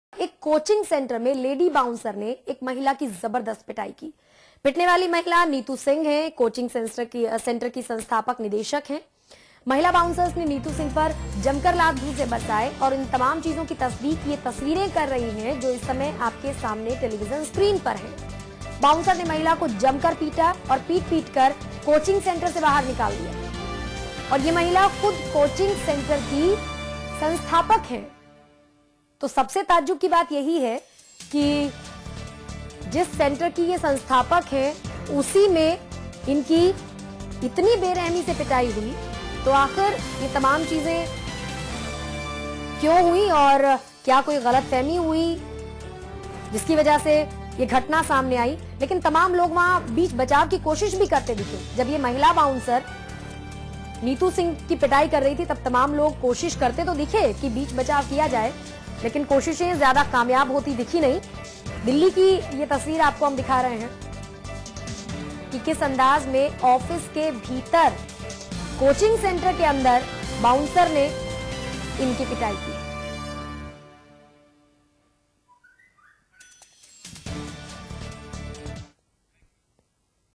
Caught on Camera: Lady bouncer beats coaching owner's wife in Delhi
New Delhi: A woman named Neetu Singh, wife of owner of Paramount coaching went to meet her husband at the center but lady bouncer stopped her and didn't allow to meet. After which they both got involved in a heated fight.